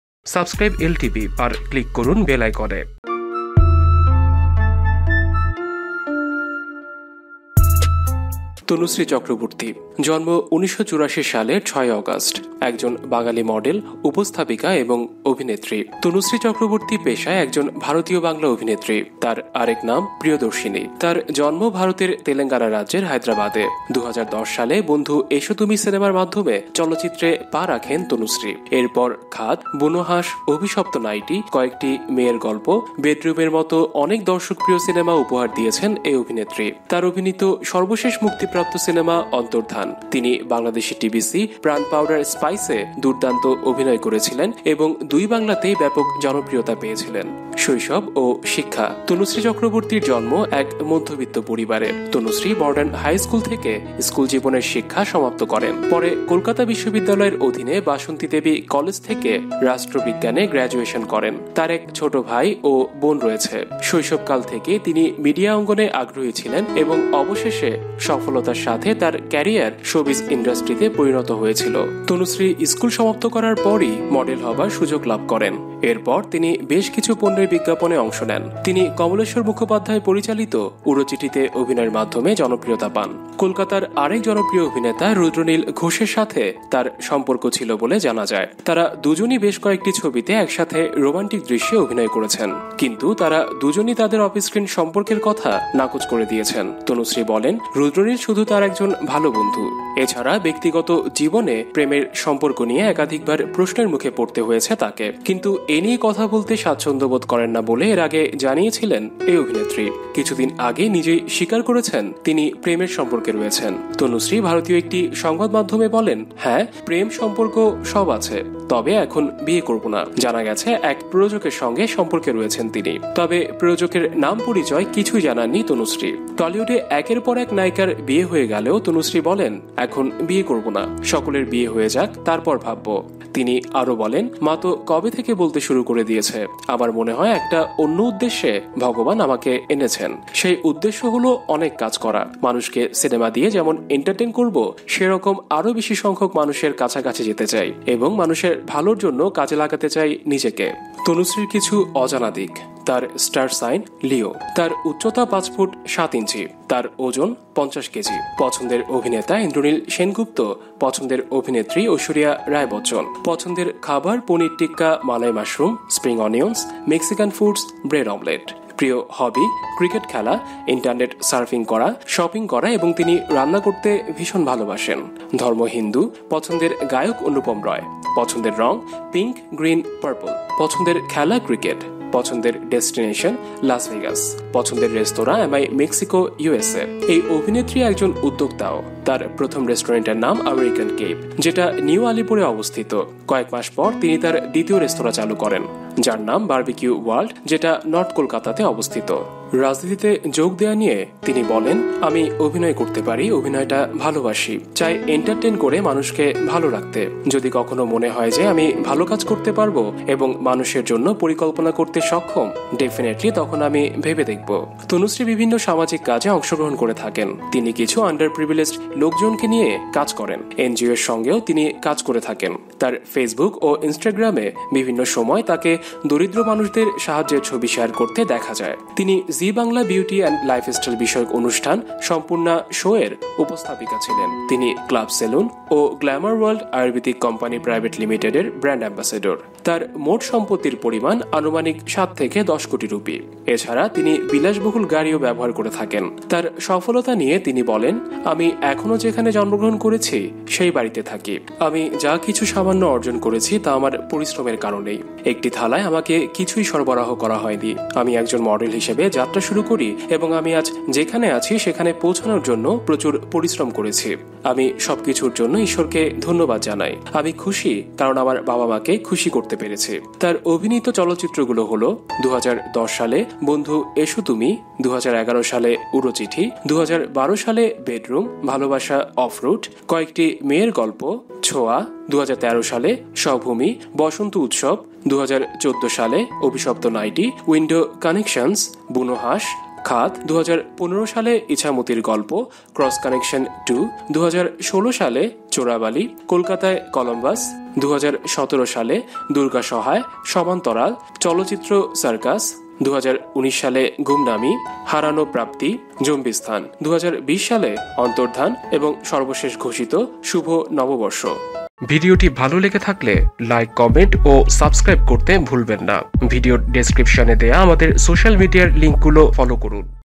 নায়িকা তনুশ্রী চক্রবর্তী এর জীবন কাহিনী | Biography of Indian Actress Tanusree Chakraborty!
Tanusree Chakraborty is a Bengali model and actress in film and television. Chakraborty came into the limelight with performances in Bengali films such as Uro Chithi, Bedroom, Obhishopto Nighty, Window Connections, Buno Haansh Khaad and Gumnaami.

Biography of Indian Actress TanusreeChakraborty!